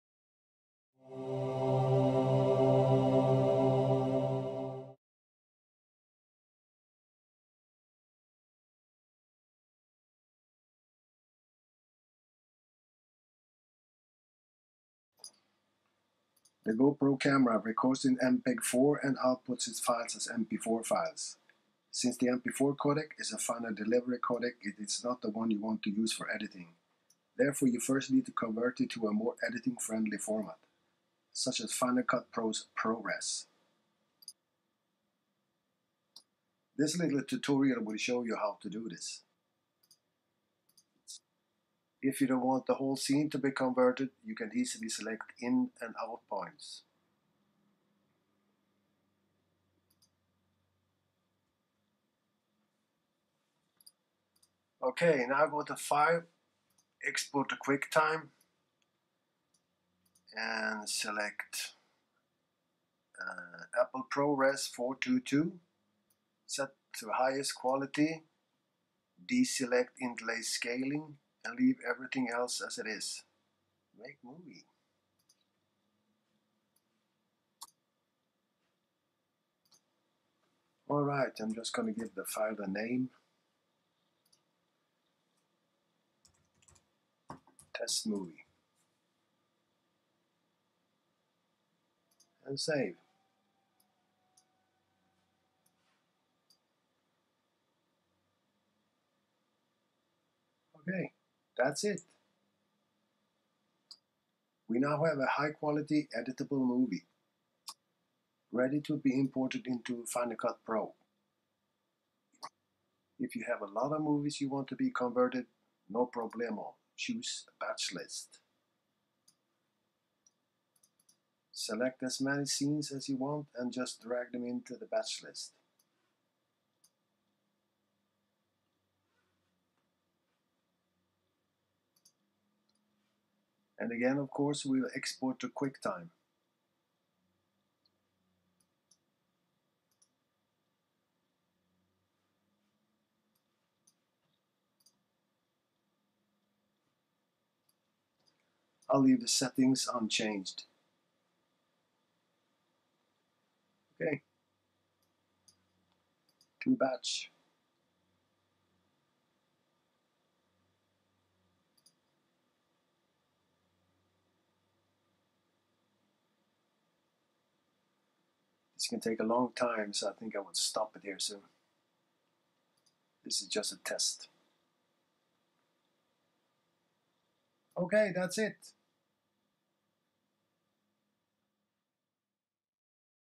Convert GoPro MP4 files to Apple Pro Res 422
Since the GoPro HD cameras MP4 files is a final delivery codec it´s not one you want to use for editing. You need to convert it to a more editing friendly format, such as Final Cut Pro´s ProRes 422. The free very powerful application MPEG streamclip does the job perfectly. Sorry about the poor audio quality in this video.